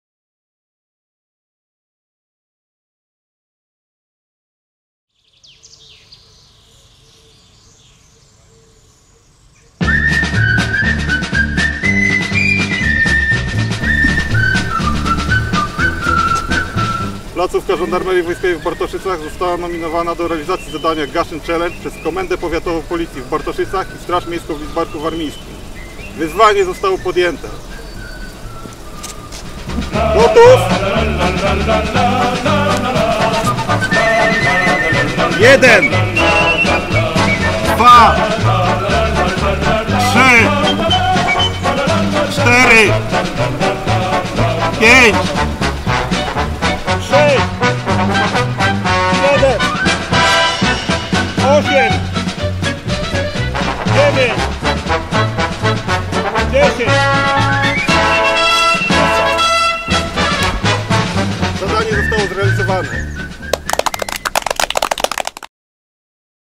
GaszynChallenge - Placówka ŻW w Bartoszycach
Żołnierze Placówki Żandarmerii Wojskowej w Bartoszycach włączyli się do akcji Gaszyn Challenge.